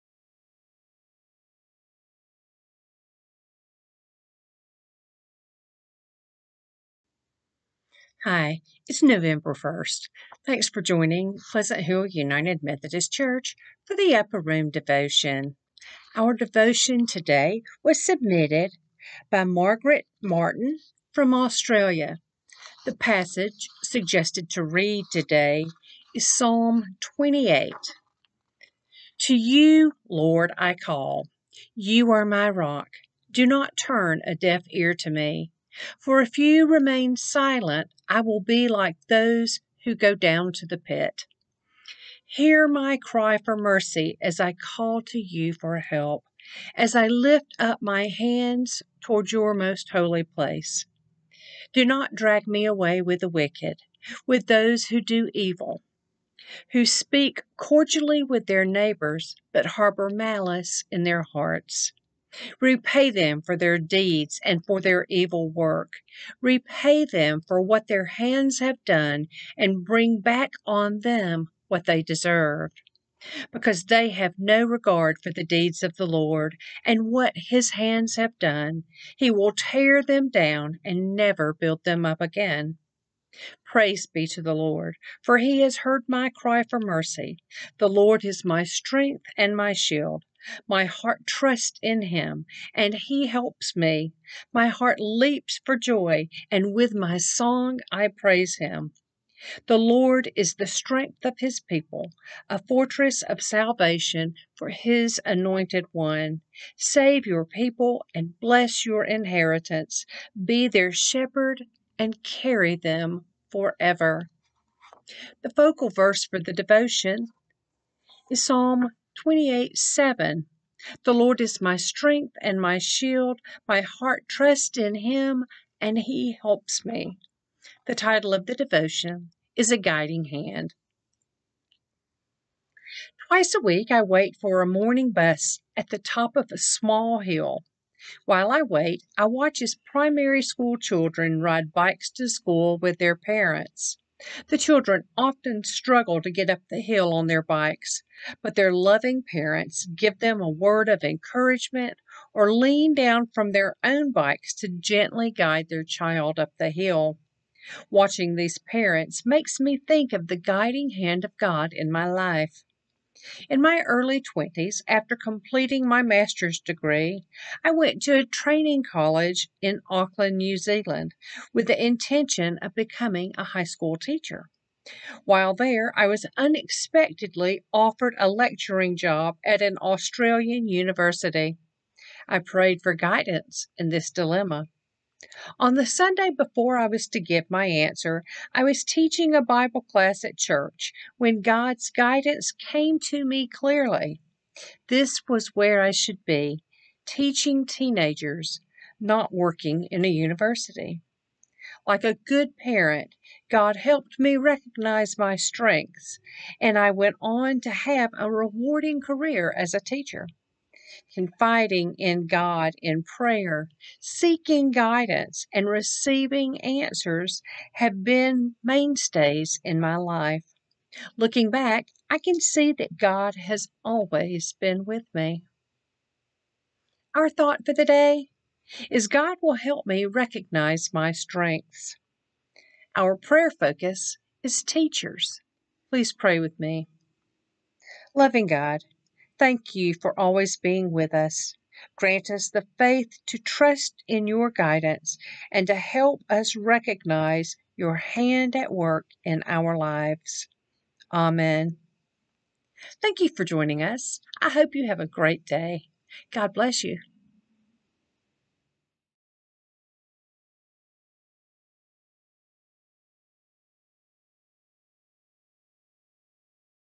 An Upper Room Devotional with Barbara Mosley for 11/1/21
Join Rev. Barbara Hayes Mosley for an Upper Room reading and devotional for today, Monday, November 1, 2021.

You can also mail your gifts to the church office:
Pleasant Hill UMC
4809 Bell Hill Road • Bessemer, AL 35022.